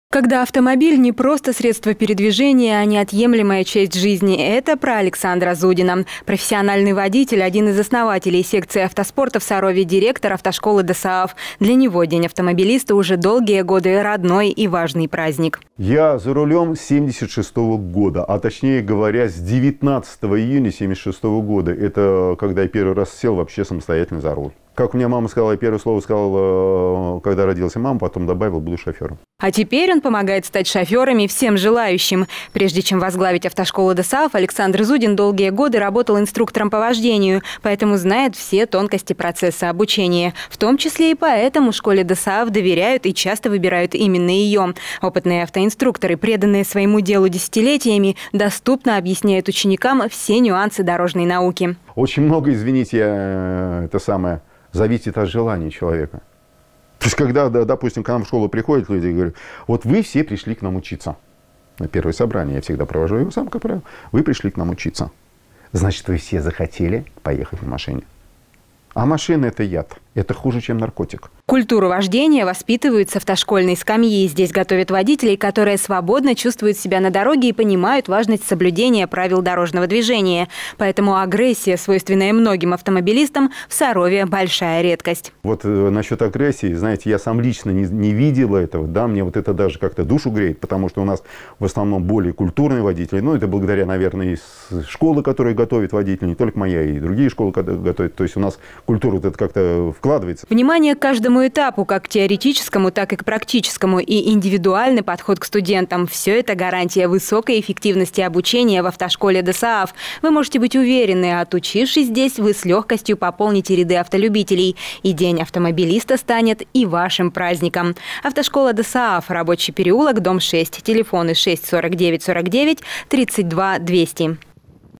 Как выбрать автошколу?
В последнее воскресенье октября в России отмечают День автомобилиста. Тем, кто пока только мечтает получить водительские права, нужно ответственно подойти к выбору автошколы.